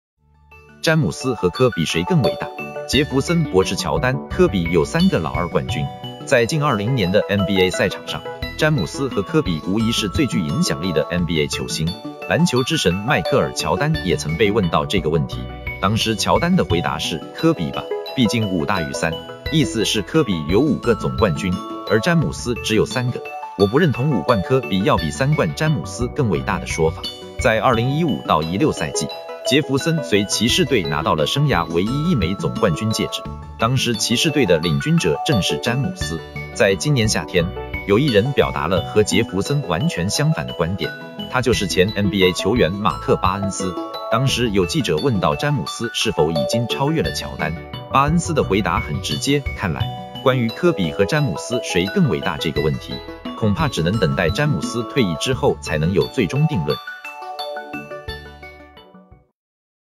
詹姆斯和科比谁更伟大？杰弗森驳斥乔丹：科比有3个老二冠军
➤Thanks收看！